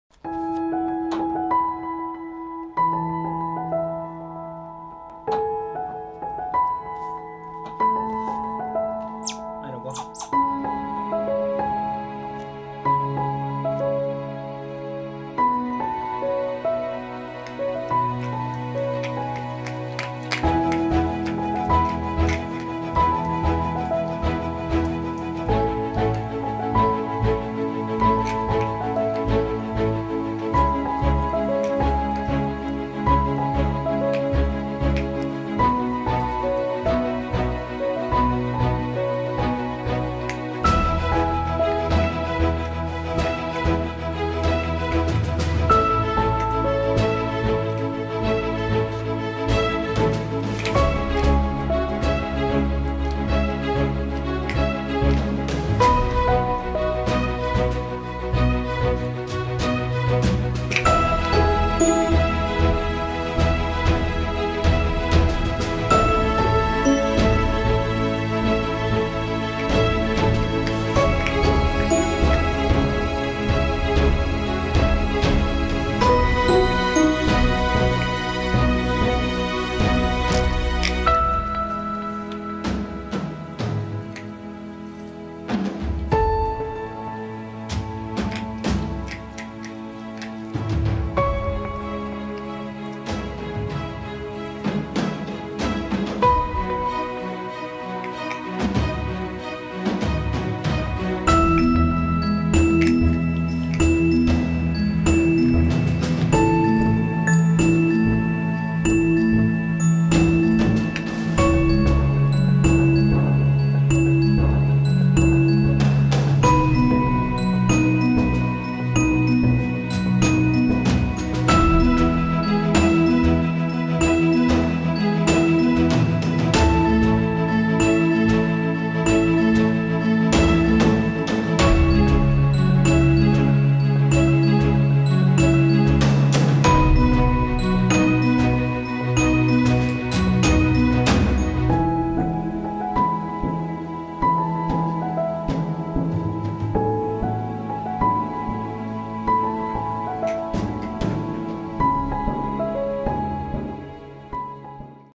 Eat food // Otter Film #2 // (ごはん カワウソのマイロ)
コツメカワウソのマイロ、ごはんの時間です。
手を器用に使っておいしそうに食べてくれます。

Hello! My name is milo.  Born in Kotsumate in Japan!
こんにちは！カワウソカフェ「コツメイト」さん出身のマイロと申します！

父ちゃんは本職で映像クリエイターをやっております。